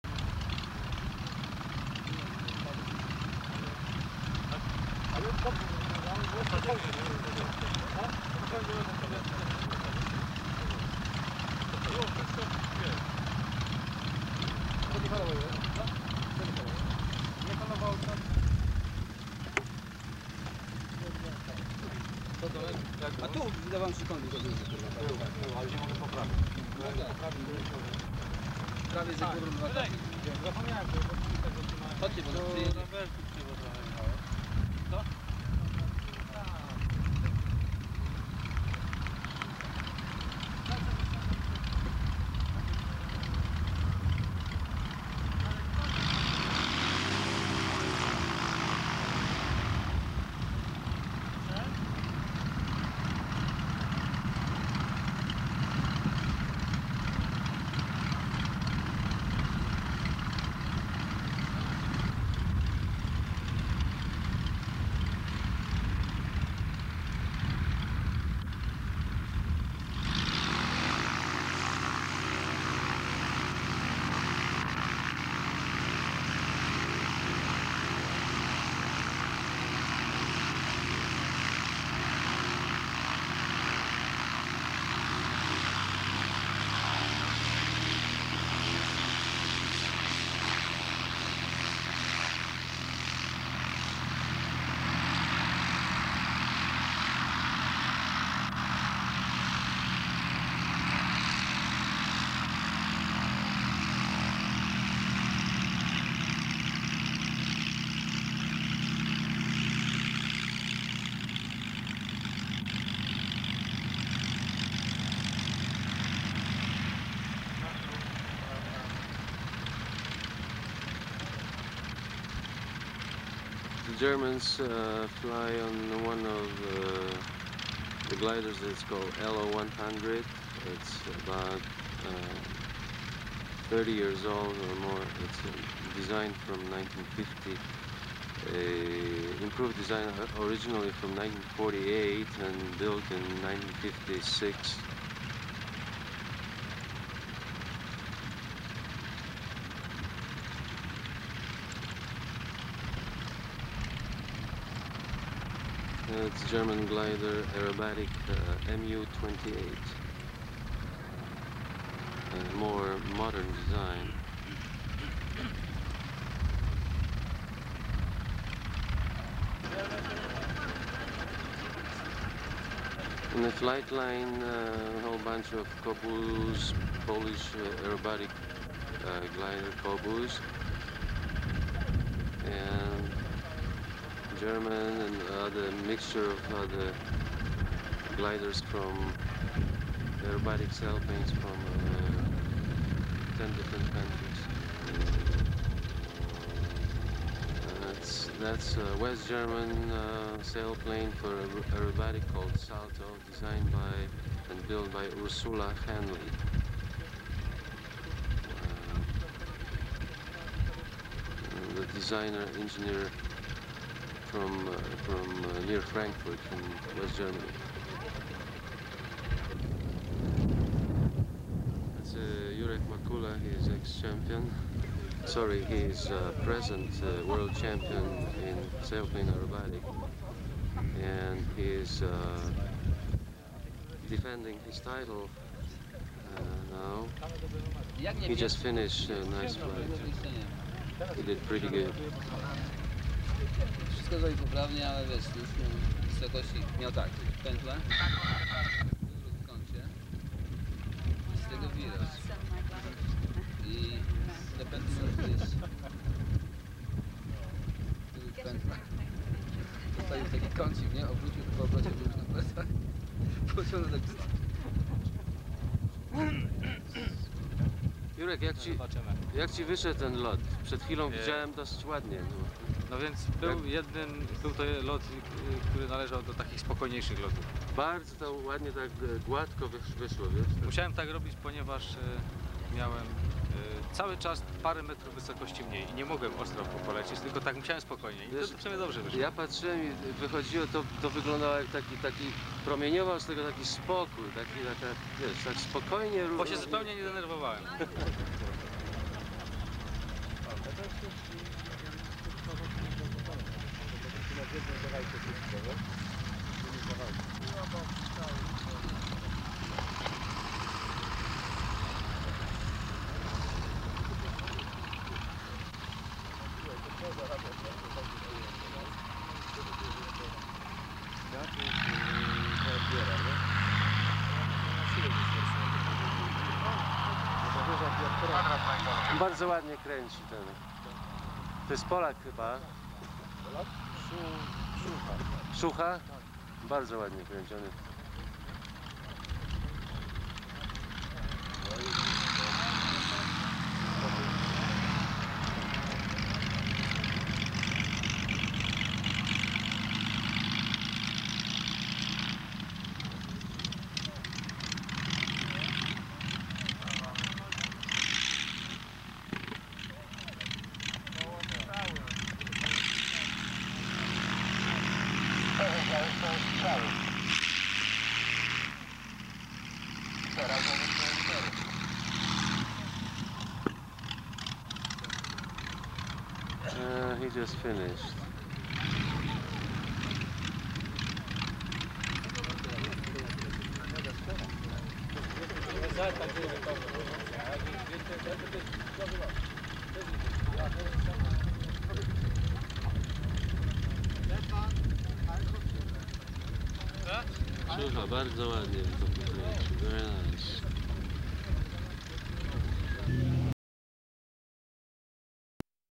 World Championships 1987, Marek Szufa flying Kobuz-3 in Bielsko
1987 video, Marek Szufa is flying SZD-21 Kobuz 3, representing Poland during 1987 Glider Aerobatic World Championships in Bielsko-Biala. Jerzy Makula (7 times world  champion), Krzysztof Wyskiel, Andrzej Tomkowicz and Józef Solski are also on this video.
Glider SZD-21 Kobuz-3 story, specifications, photos:
Andrzej Tomkowicz parachuted from Kobuz when the wings failed during training flight in 1989. Krzysztof Wyskiel was killed when the wings failed in 1991 at the world glider aerobatic championships at Hockenheim in Germany . SZD glider factory test pilot Sławomir Makaruk was killed when Kobuz broke up in flight during flutter testing in 1963.